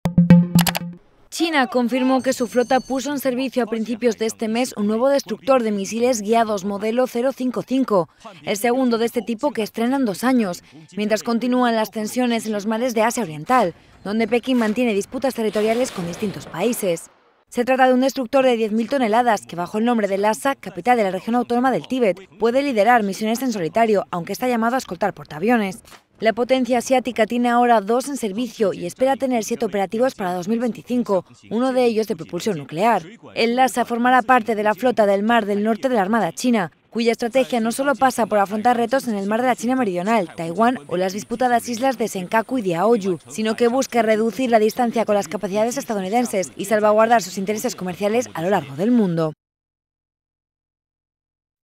China estrena destructor de misiles en un momento de tensiones en mares de Asia oriental
China confirmó hoy que su flota puso en servicio a principios de este mes un nuevo destructor de misiles guiados modelo 055, el segundo de este tipo que estrena en dos años, bajo un contexto de tensiones en los mares de Asia oriental, donde Pekín mantiene disputas territoriales con distintos países.

¡Visítenos!